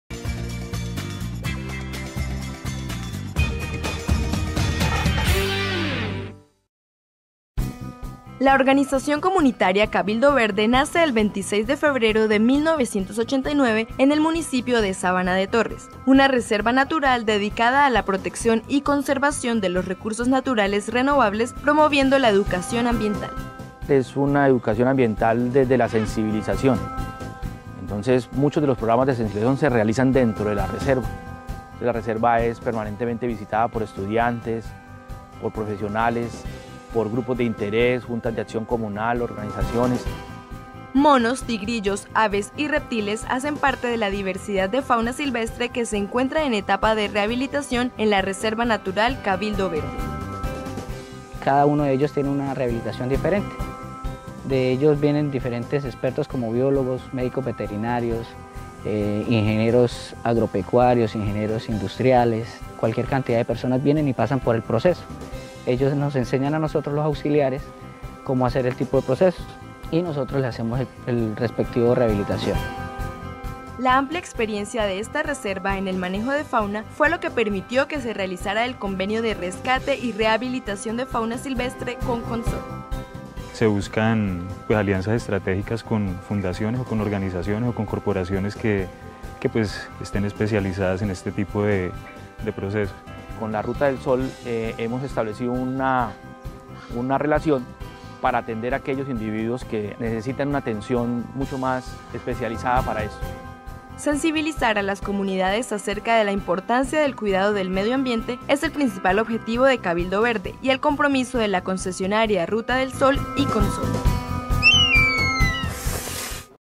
La historia de la ONG Cabildo Verde
Lo invitamos a conocer sobre la historia de la ONG Cabildo Verde, que en convenio con CONSOL rehabilita y devuelve a su hábitat natural los animales encontrados en la Ruta del Sol Sector 2.